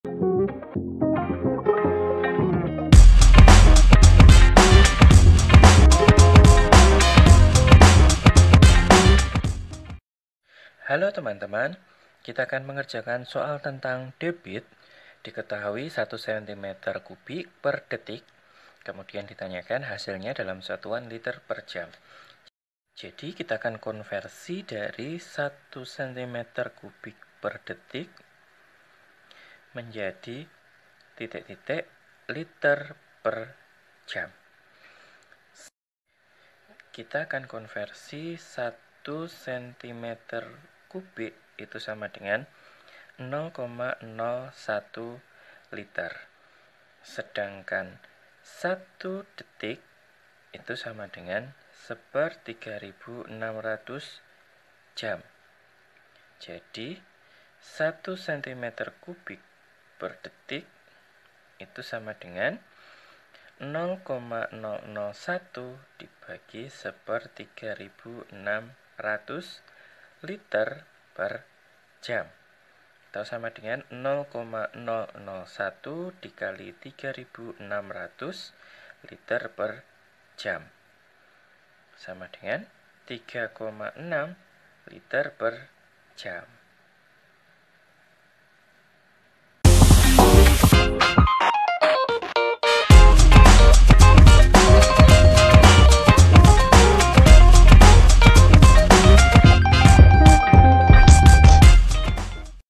1 \mathrm{~cm}^{3} / \mathrm{det}=\ldots \... | Gemar Matematik | Kelas 6 | Matematika
✅ Dalam video ini kita akan membahas: 1 \mathrm{~cm}^{3} / \mathrm{det}=\ldots \text { liter/jam }
Tema:
Kelas: 6
Mata Pelajaran: Matematika
Bab: 3
Buku: Gemar Matematika 6 Untuk Kelas VI SD/MI

👉 Buku yang telah tersedia:
{PLAYLIST_LINK}
Di mana kamu dapat menemukan kami?